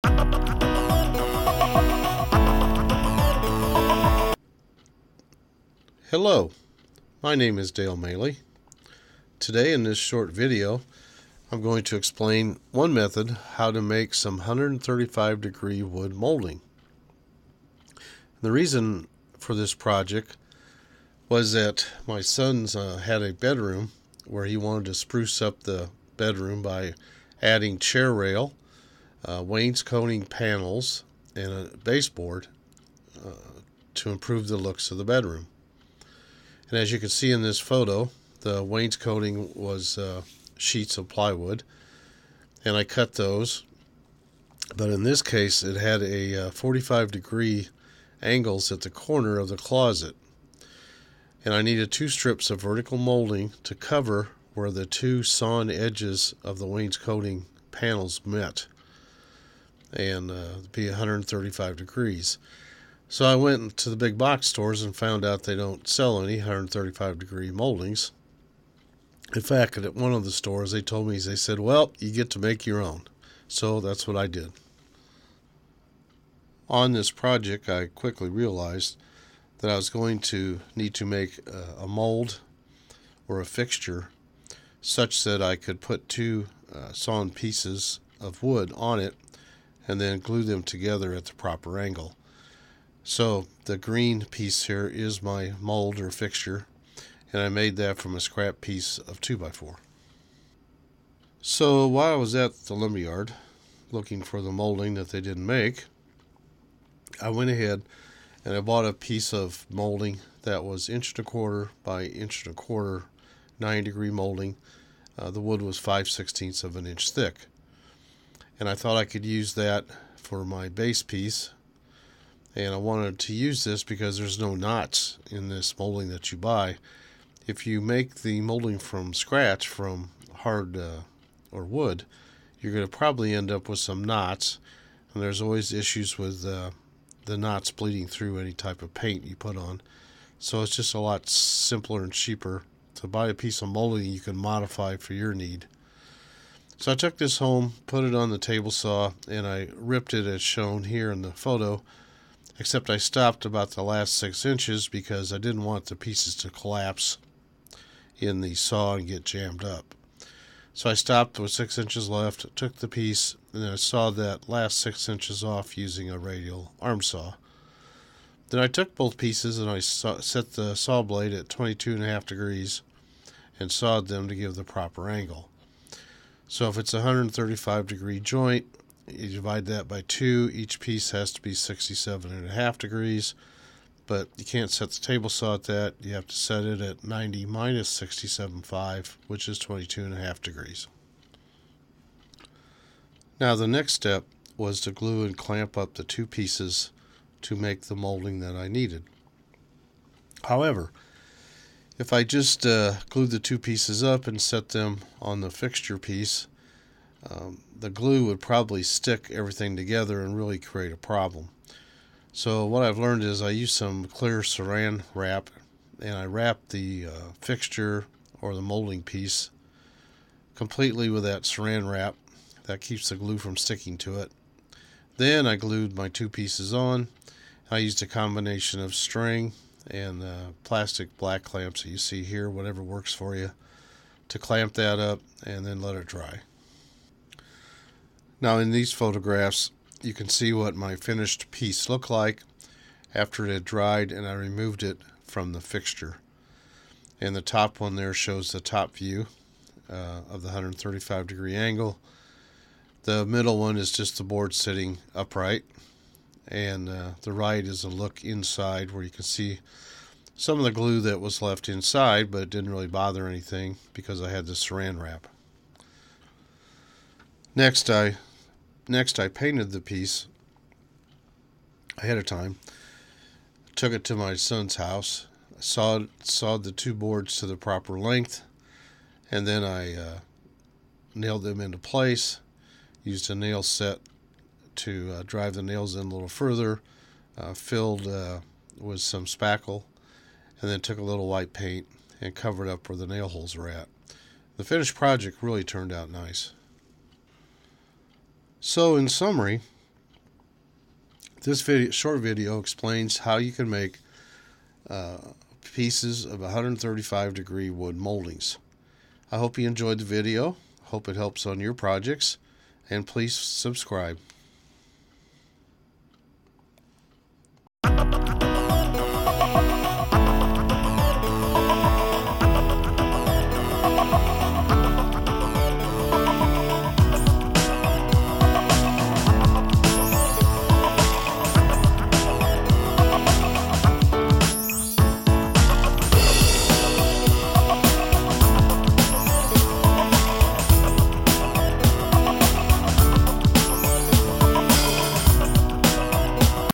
How to Make 135 Degree wood moldings
Explains how to make pieces of 135 degree (or 22.5 degrees on table saw) to fit on 45 degree corners of rooms.